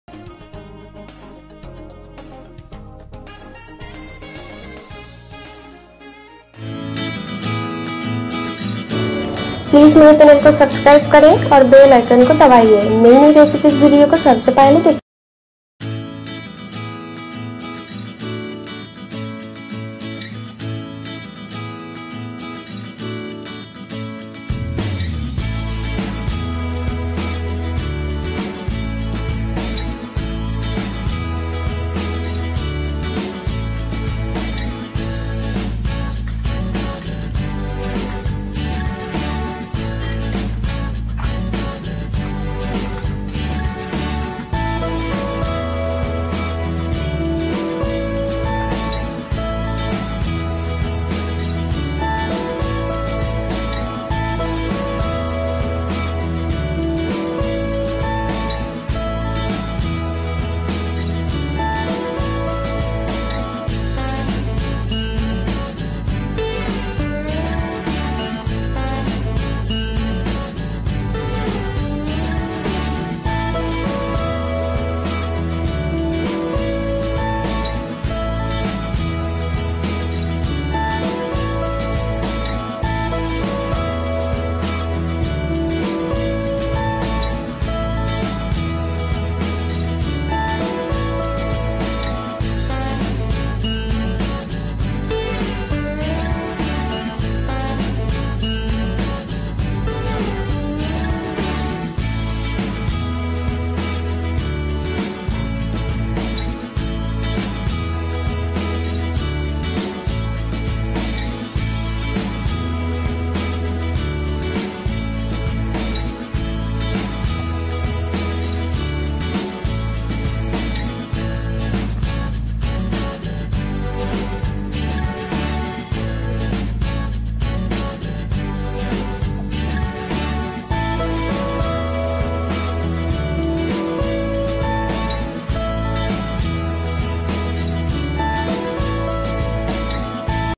Fish Egg Fry | Bengali Macher Dimer Bora | Easy Fish Eggs Pakora Recipe
Fish eggs pakora is famous recipe in kolkata.Today show you how to make bengali macher dimer bora easy preparation.Macher dimer pakora is a Bengali side dish recipe usually cooked in rainy season when fish lay their egg.Macher dim is good source of protein. This recipe can be served with evening tea or as a starter with lunch.Serve while hot with salad and chutney.
For Buy Essential Home and Kitchen Items